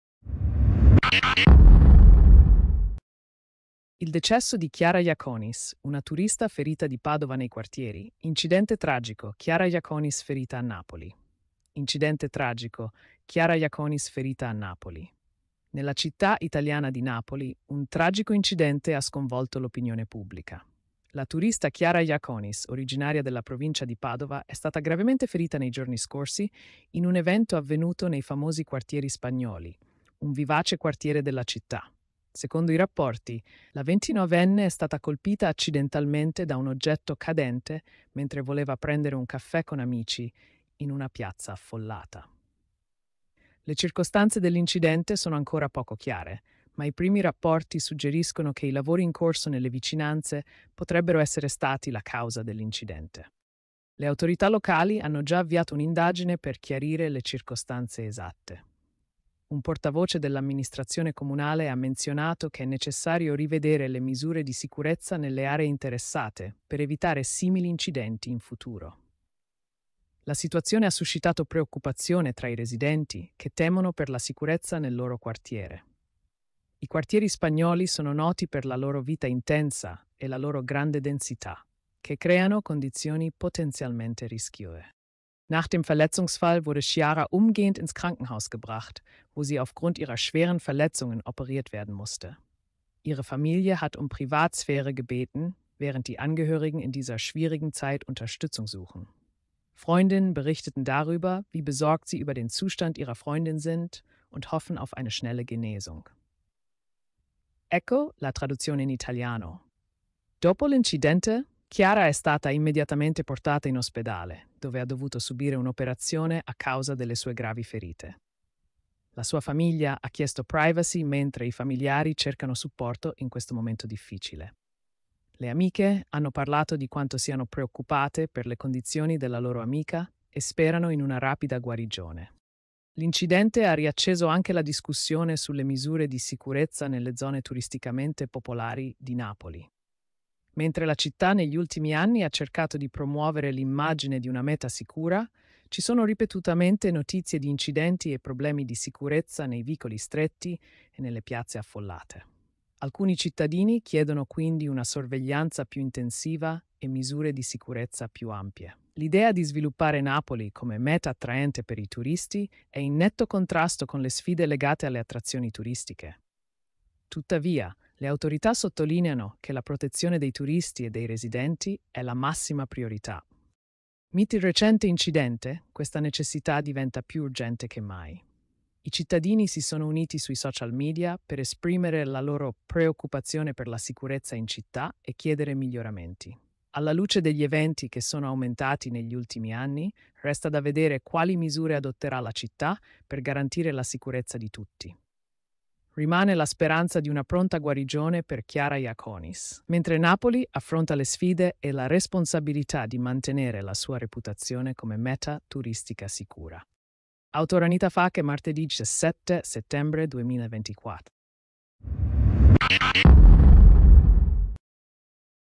Mortale incidente della turista Chiara Jaconis a Napoli. (Podcast)
A Napoli, la 29enne Chiara Jaconis è stata gravemente ferita. [29562] [AI-generated content]